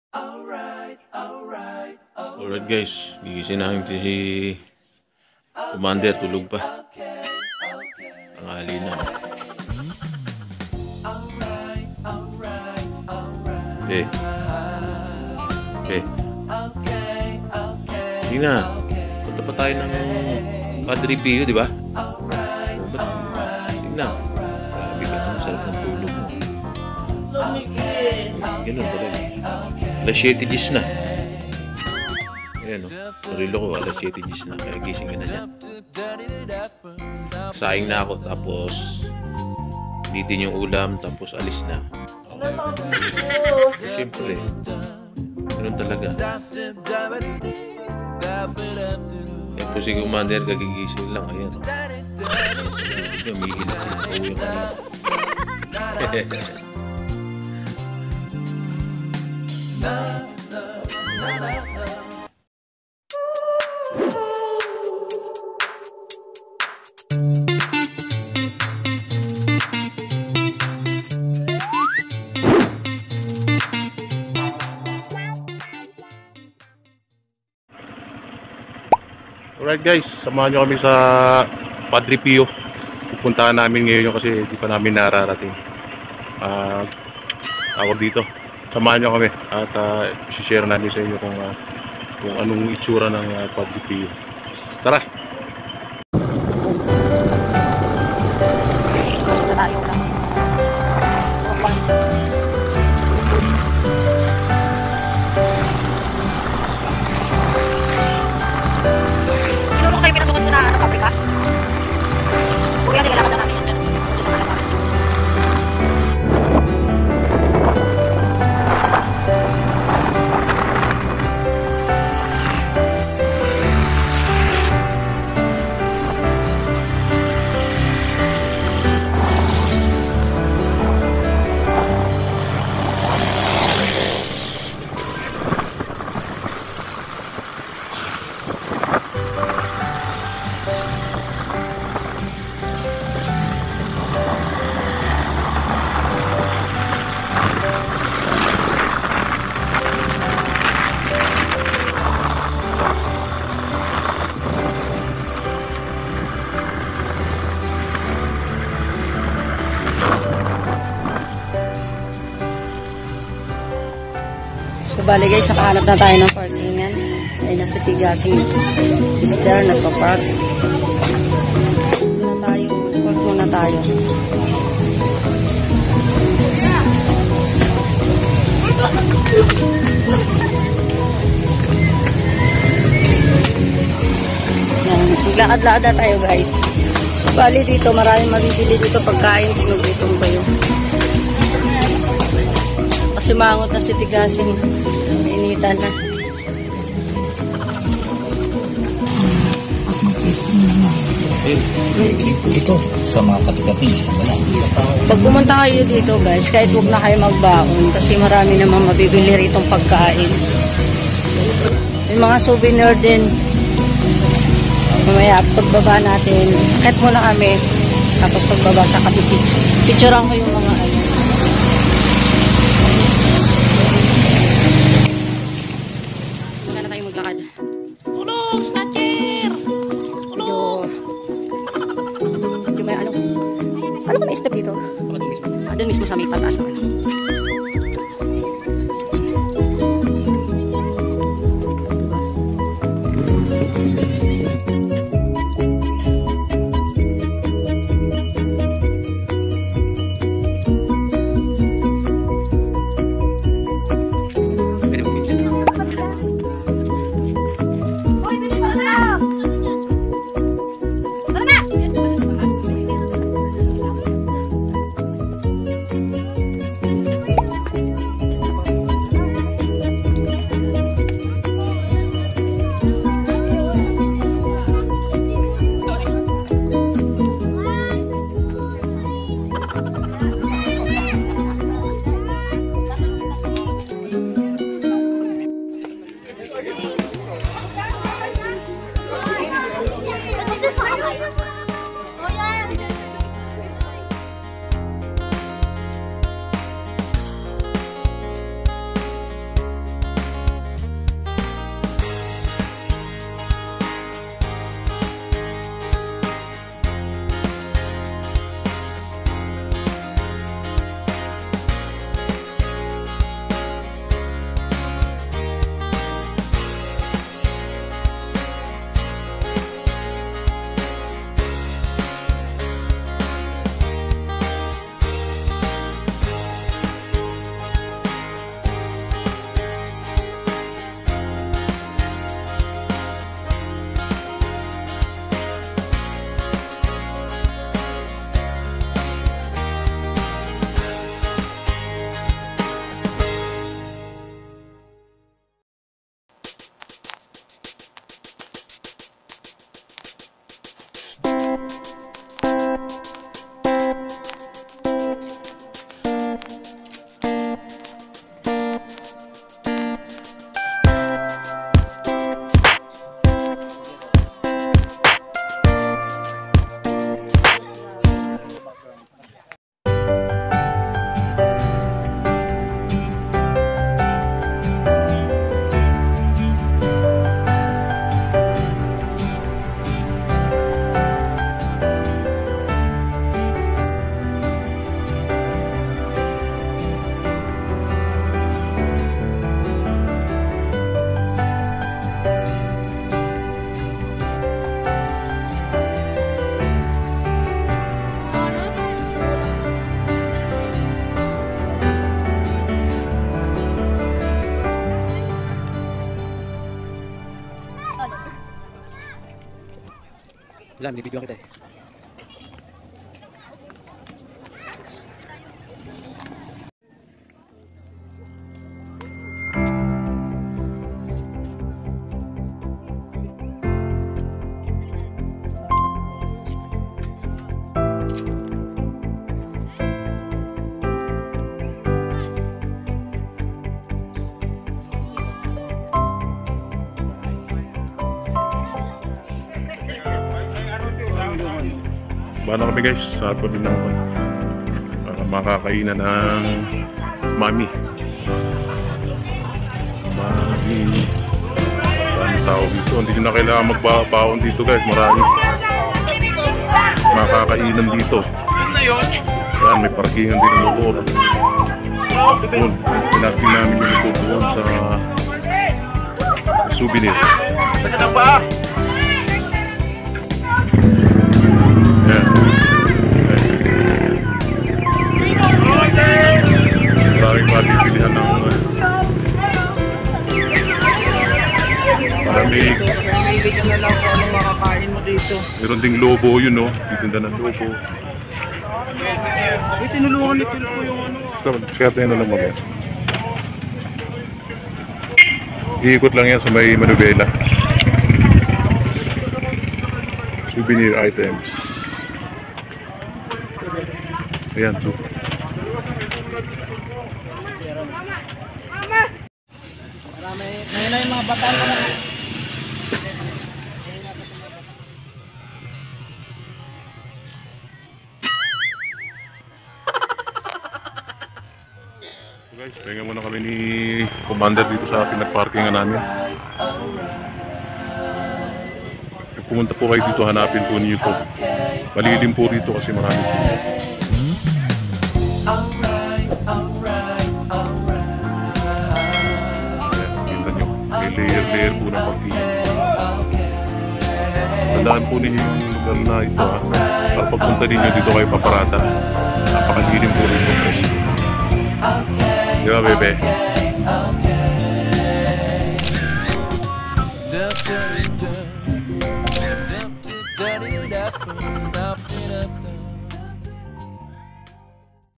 Padre Pio vlog | Padre Pio Shrine | Tigasing Mister @ Commander
samahan niyo kami sa Padre Pio shrine sa Bulacan at saksihan ang makasaysayang ang kagandahan nito. sa mga gsto mapunta ihanda ang sarili dahil matarik ang daanan patungo sa mismong higanteng rebulto. mag dasal at mag pasalamat sa Panginoong Jesukristo. Salamat at God bless you all.